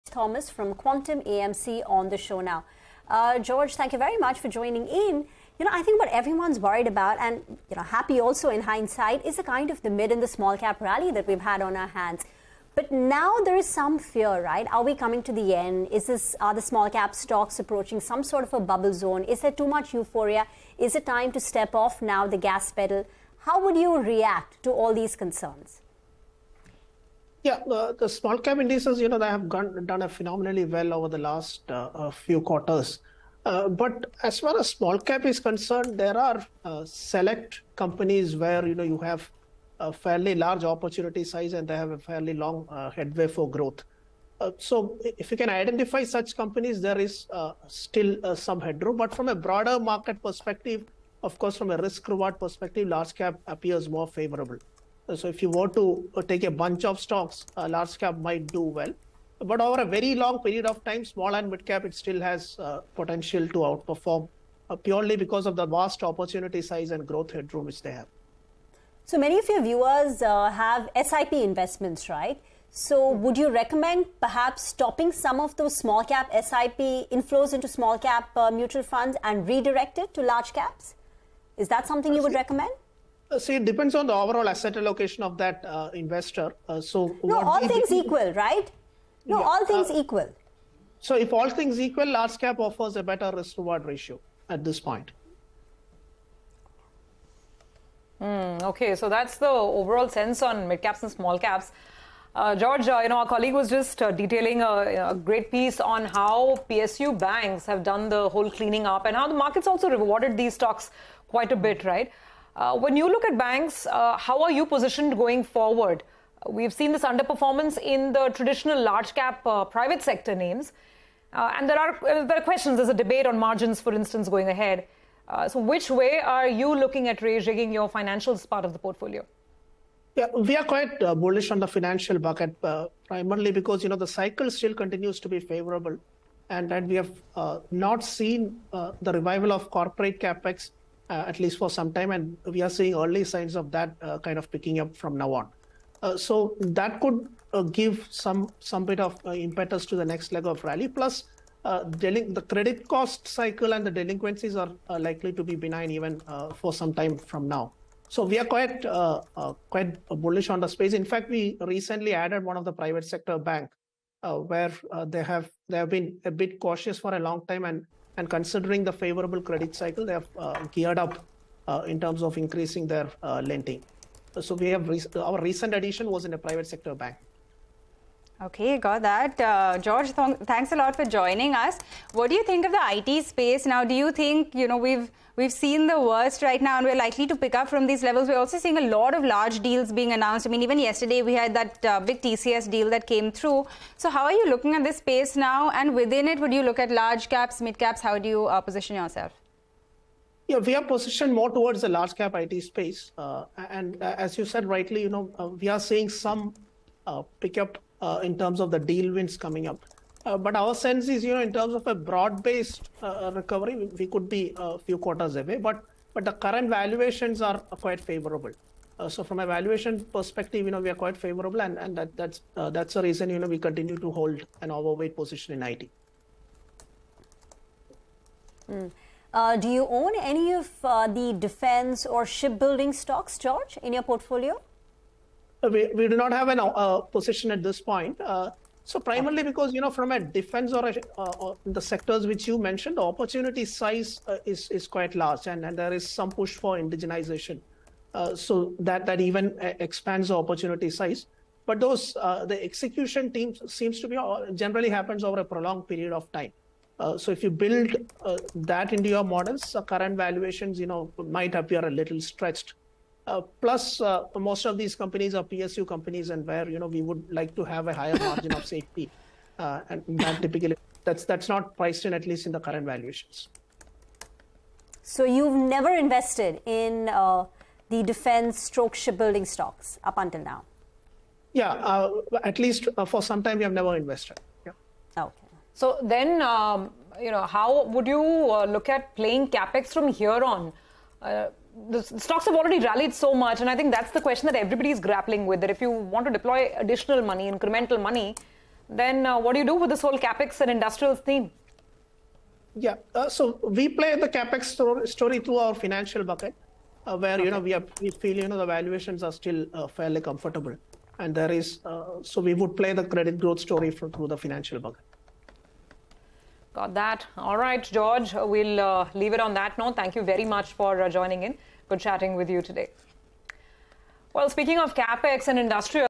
Small Caps Have Done Very Well, But Only A Select Few Have A Fairly Large Capacity: Quantum AMC
Market Outlook | Small cap indices have done very well over the last few quarters, but only a select few have a fairly large capacity, says George Thomas of QuantumAMC. Tells @PavitraParekh1 @Reematendulkar & @SurabhiUpadhyay large caps appear more favourable.

n18oc_business